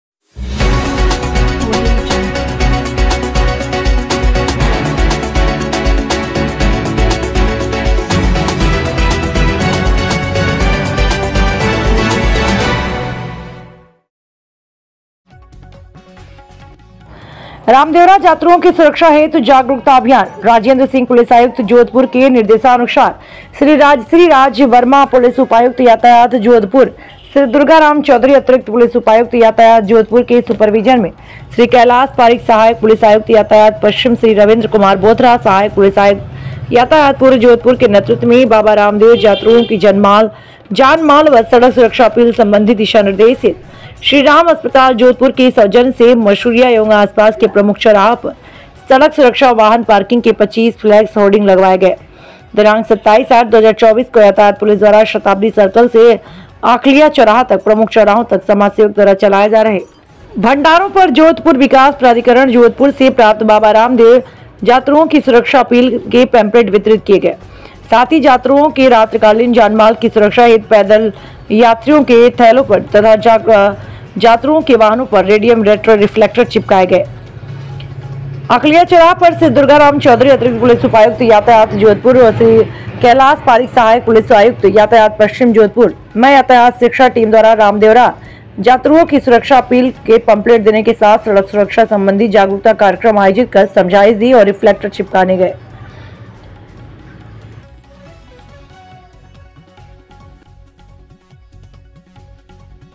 रामदेवरा जातरूओं की सुरक्षा हेतु जागरूकता अभियान
रिपोर्टर आशीष पुरोहित
जोधपुर/ राजस्थान

जोधपुर। राजेन्द्रसिंह पुलिस आयुक्त जोधपुर के निर्देशानुसार श्री राजर्षि राज वर्मा पुलिस उपायुक्त यातायात जोधपुर, श्री दुर्गाराम चौधरी अतिरिक्त पुलिस उपायुक्त यातायात जोधपुर के सुपरविजन में श्री कैलाश पारीक सहायक पुलिस आयुक्त यातायात पश्चिम, श्री रविन्द्र कुमार बोथरा सहायक पुलिस आयुक्त यातायात पूर्व जोधपुर के नेतुत्व में बाबा रामदेव जातरूओं की जानमाल एवं सड़क सुरक्षा अपील संबंधित दिशा-निर्देश हेतु श्रीराम अस्पताल जोधपुर के सौजन्य से मसुरिया एवं आसपास के प्रमुख चौराहों पर सड़क सुरक्षा एवं वाहन पार्किंग के 25 फ्लेक्स हॉर्डिंग लगावाये गये। आज दिनांक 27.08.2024 को यातायात पुलिस द्वारा शताब्दी सर्कल से आखलिया चौराहा तक प्रमुख चौराहों तथा समाजसेवकों द्वारा चलाये जा रहे भण्डारों पर जोधपुर विकास प्राधिकरण जोधपुर से प्राप्त बाबा रामदेव जातरूओं की सुरक्षा अपील के पेम्पलेट वितरित किये गये तथा साथ ही जातरूओं की रात्रिकालीन जानमाल की सुरक्षा हेतु पैदन यात्रियों के थैलों पर तथा जातरूओं के वाहनों पर रेडियम रेट्रों रिफ्लेक्टर चिपकाये गये।

आखलिया चौराहा पर श्री दुर्गाराम चौधरी अतिरिक्त पुलिस उपायुक्त यातायात जोधपुर

व श्री कैलाश पारीक सहायक पुलिस आयुक्त यातायात पश्चिम जोधपुर मय यातायात शिक्षा टीम द्वारा रामदेवरा जातरूओं की सुरक्षा अपील के पेम्पलेट देने के साथ सड़क सुरक्षा संबंधी जागरूकता कार्यकम आयोजित कर समझाईश की गई व रिफ्लेक्टर चिपकाने गये। रामदेवरा जातरूओं की सड़क सुरक्षा एवं जागरूकता के इसी क्रम में यातायात शिक्षा टीम द्वारा शताब्दी सर्कल, मधुबन मोड़, तथा मेडिकल चौराहा पर जाग्रूकता कार्यक्रम के साथ समझाईश की गई। यातायात पुलिस द्वारा जातरूओं की सुरक्षा अभियान मेलापर्यन्त जारी रहेगा।